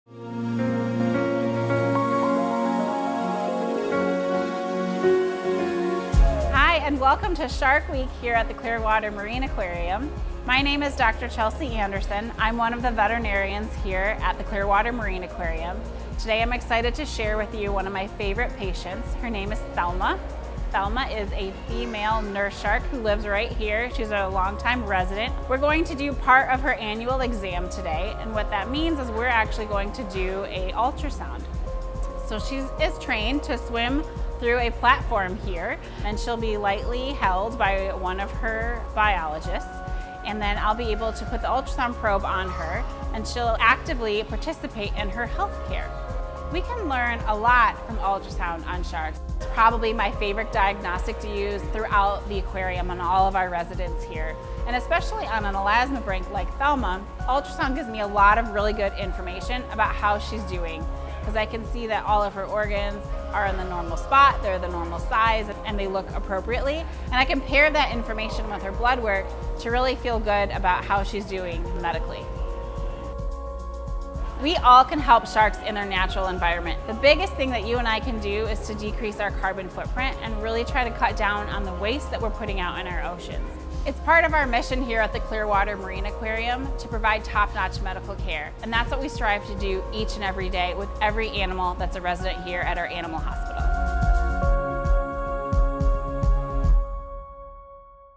Nurse Shark Ultrasound
This Shark Week, we're giving you an inside look into shark health care at CMA. Tune in as Dr. Chelsea, CMA's Senior Veterinarian, performs an ultrasound on our nurse shark Thelma.